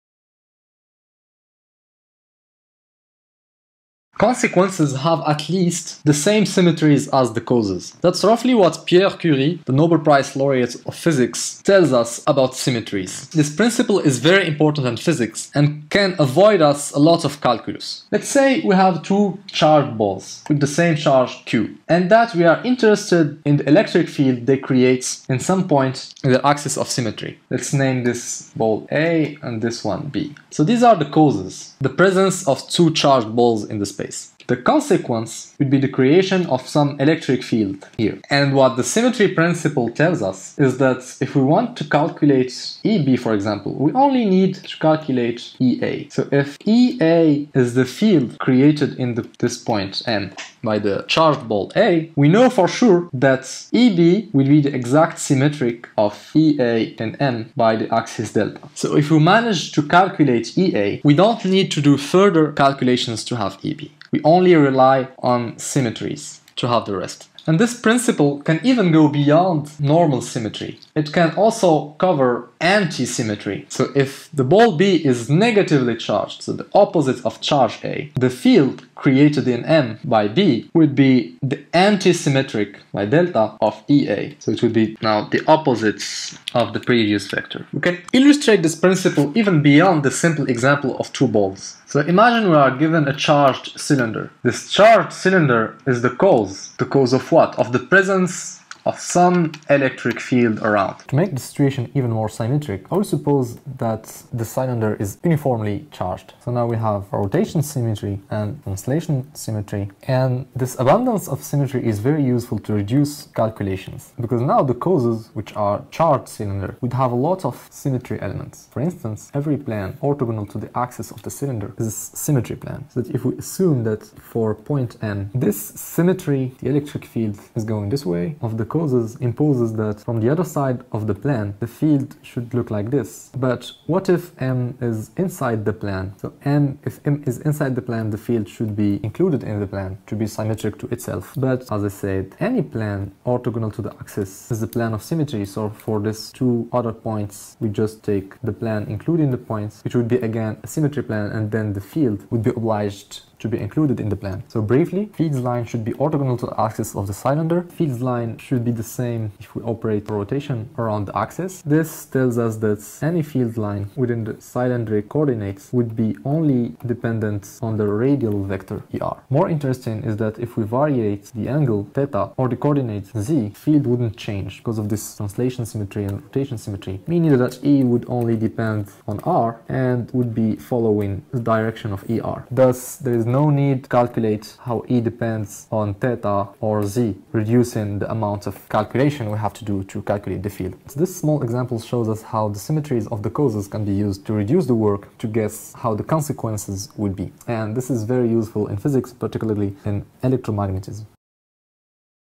Symmetry Principle in Physics | El Mahdi El Mhamdi
Symmetries are fundamental in Physics, from a practical point of view, they can help us reduce the calculations to do and focus on the understanding of the problem. Here is an example from electrostatics.
Speaker: El Mahdi EL MHAMDI.